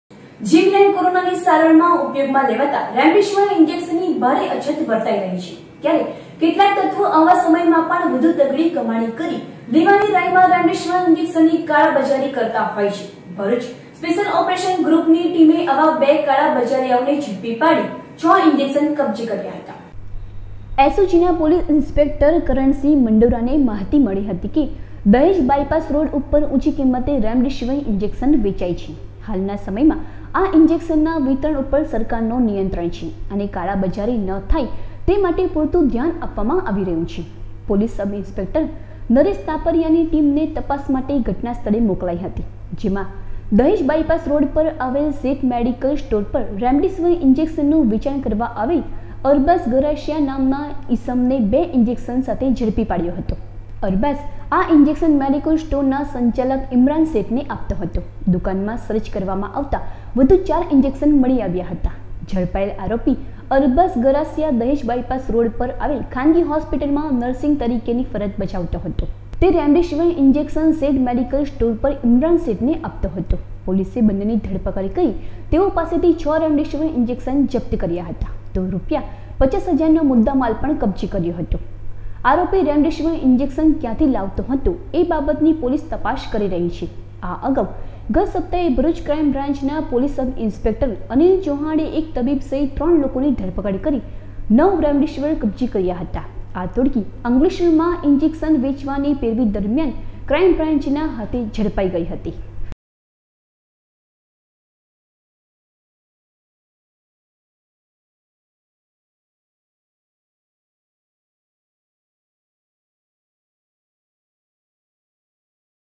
માં ટીવી 04.05.2021
કોરોના ની કાળા બજારી : સરકારે દર્દીઓના ઈલાજ માટે આપેલા રેમેડીસીવીર  ઇન્જેક્શનની કાળાબજારી કરતા આરોગ્યકર્મી સહીત બે ઝડપાયા
જીવલેણ કોરોનાની સારવારમાં ઉપયોગમાં લેવાતા રેમડેસિવિર ઈન્જેકસનની ભારે અછત વર્તાઈ રહી છે :  ત્યારે કેટલાક તત્વો આવા સમયમાં પણ વધુ તગડી કમાણી કરી લેવાની લ્હાયમાં રેમડેસિવિર ઈન્જેકસનની કાળા બજારી કરાય :   ઝડપાયેલ આરોપી પૈકી 1 રોપી દહેજ બાયપાસ ની ખાનગી હોસ્પિટલ ફરજ નિભાવે છે : 6 રેમડેસિવિર ઈન્જેકસન કબ્જે કરાયા : અગાઉ તબીબ અને મંડળી ૯ ઇન્જેક્શન સાથે ઝડપાઇ